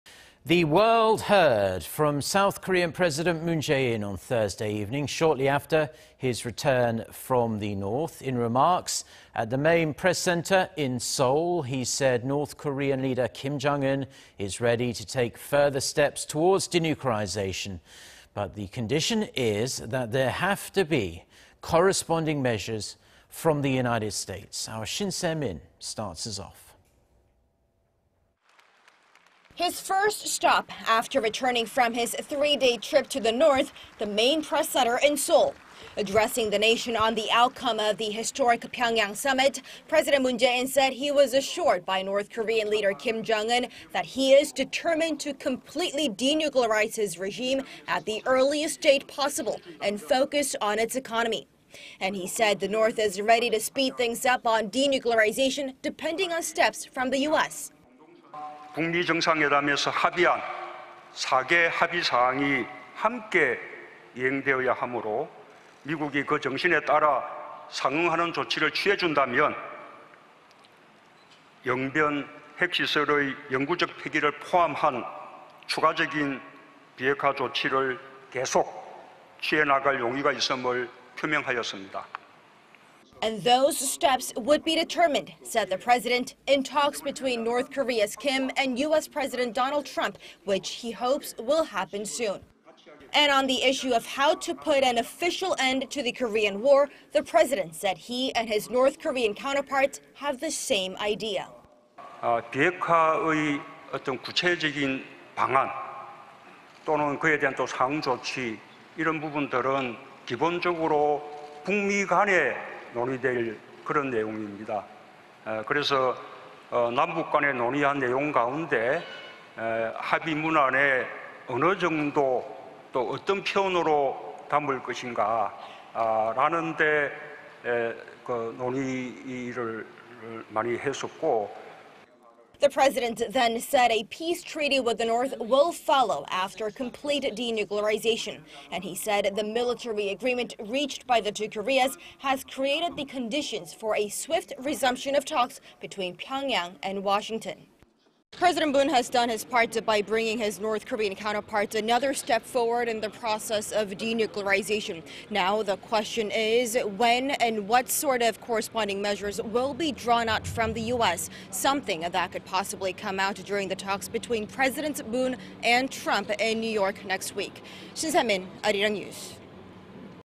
Presidential address on Pyeongyang summit: Pres. Moon says he had been assured by Kim
3차 남북정상회담 마친 문재인 대통령 대국민보고
  The world heard from South Korean President Moon Jae-in on Thursday evening... shortly after his return from the North.
In remarks at the main press center, he said North Korean leader Kim Jong-un... is ready to take further steps towards denuclearization, but the condition is that there have to be corresponding measures from the United States.
Shin Se-min reports.
  His first stop after returning from his three-day trip to the North -- the main press center in Seoul.
Addressing the nation on the outcome of the historic Pyeongyang Summit, President Moon said he was assured by Chairman   Kim that he is determined to completely denuclearize his regime at the earliest date possible and focus on its economy.
And he said the North is ready to speed things up on denuclearization depending on steps from the U.S.

   "Chairman Kim has expressed his willingness to take further steps towards denuclearization once the U.S. agrees to take corresponding measures. This is because the four-point declaration agreed at the Singapore summit needs to be carried out all together."

  And those steps would be determined, said the President, in talks between Chairman Kim and Trump, which he hopes will happen soon.
On the issue of how to put an official end to the Korean War, the President said he and his North Korean counterpart have the same idea.

   "A detailed plan for denuclearization and corresponding measures are issues that need to be discussed between North Korea and the U.S. So, we gave a lot of thought to the wording we would use in the agreement we reached."

  The President then said a peace treaty with the North will follow after complete denuclearzation.
And he said the military agreement reached by the two Koreas… has created the conditions for a swift resumption of talks between Pyeongyang and Washington.

   "So President Moon has done his part by bringing his North Korean counterpart another step forward in the process of denuclearization.
The question now is when and what kind of corresponding measures will be announced by the U.S.,... possibly something that could happen during the talks between Presidents Moon and Trump next week in New York.